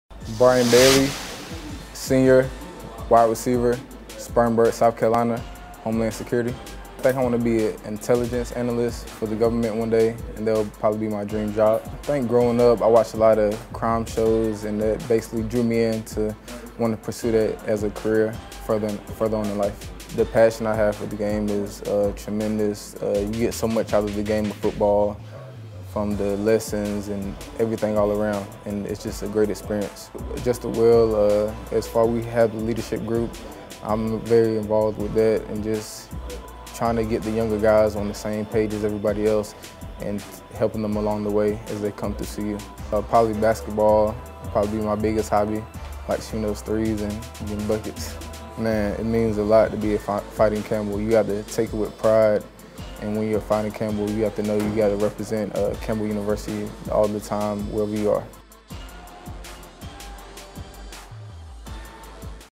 Get To Know Your Camels - Bryan Bailey
RS Senior WR Bryan Bailey brings his veteran presence to the Fighting Camels receiver corp.